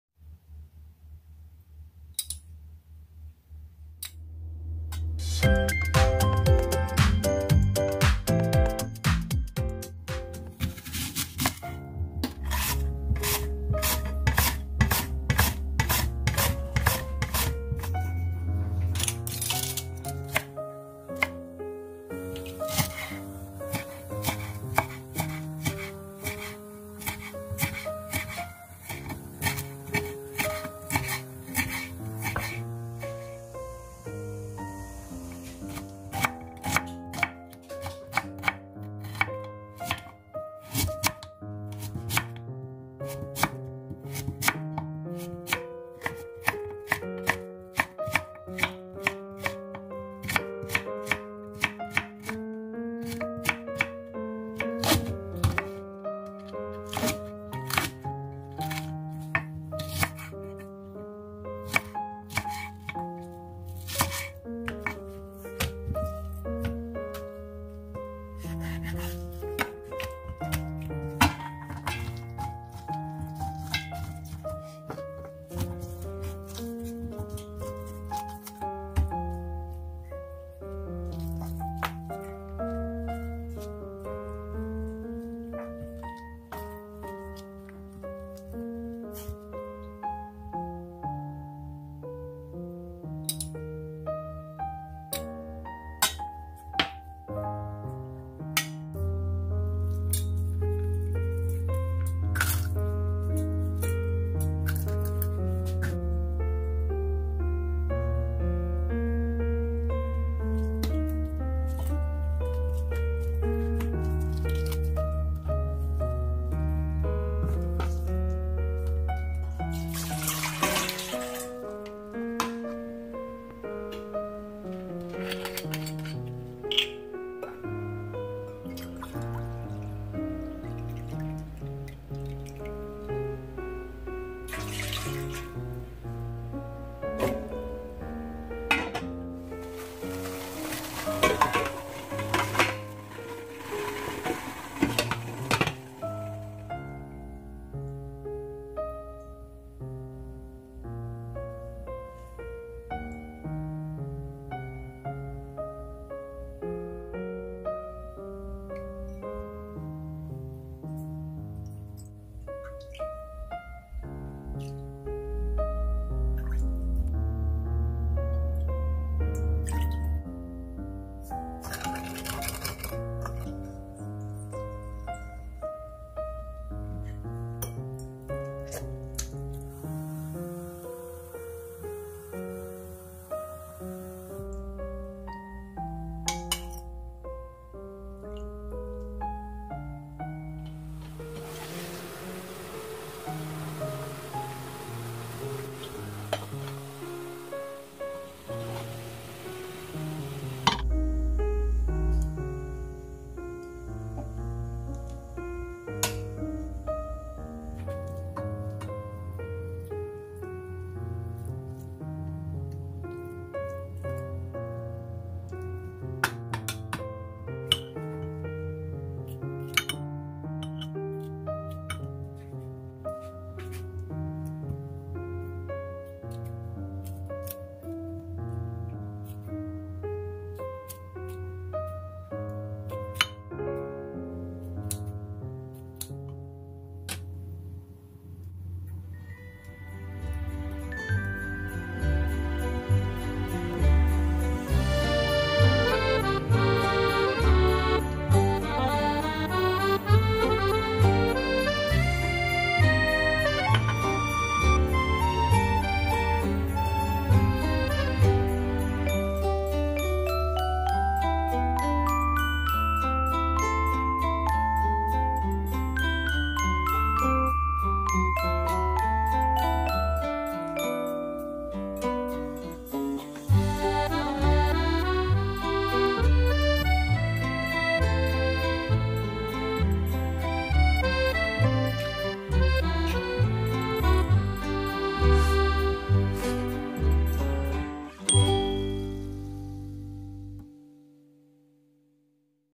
삼겹살 건강하게 먹는 방법❗삼겹살 굽지 마세요ㅣ 양파절임 레시피
♣ 삼겹살찜

✦재료
삼겹살 500g
양파 1개, 대파 2대
브로콜리, 무

생강, 굵은소금, 후추

✦양파절임
물 2큰술, 간장 2큰술
식초 2큰술, 맛술 1큰술
설탕 1큰술 , 겨자 1/2작은술

ㅡㅡ ㅡㅡ ㅡㅡ ㅡㅡ ㅡㅡ